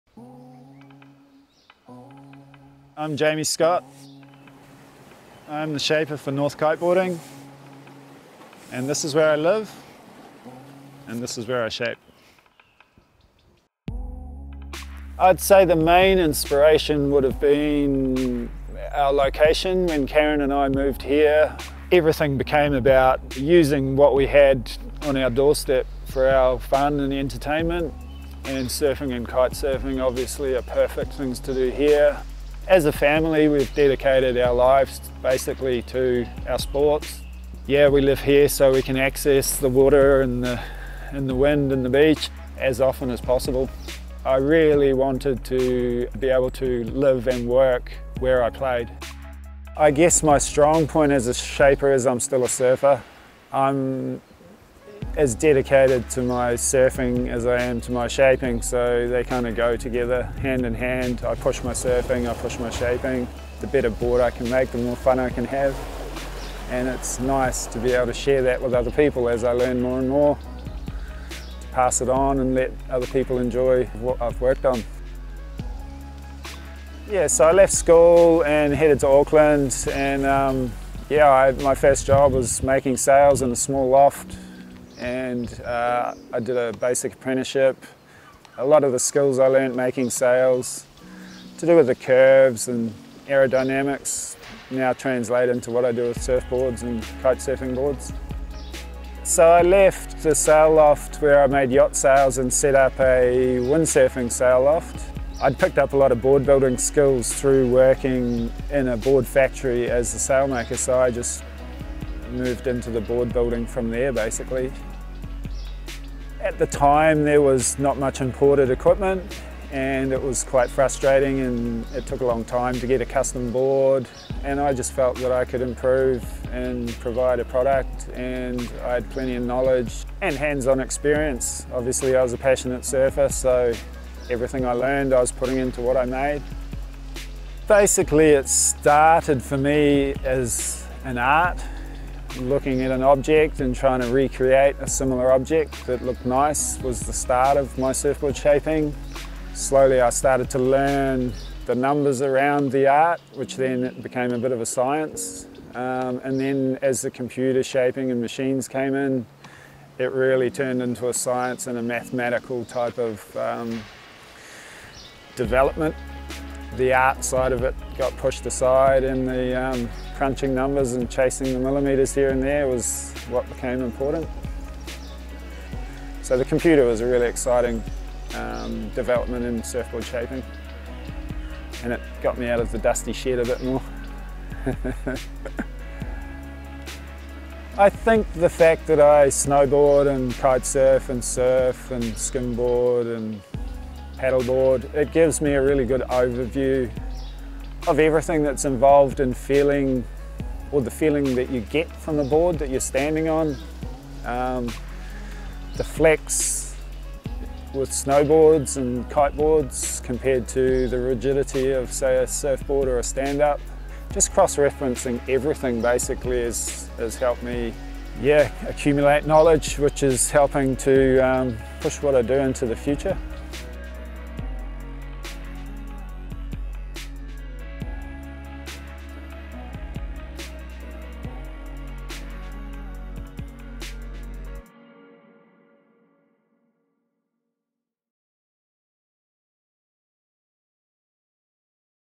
Jaimie Scott - North Surfboard Shaper Documentary
North Kiteboarding have just released a series of beautifully shot and evocative, nature inspired videos about their shaper, Jaimie Scott, his background and his shapes.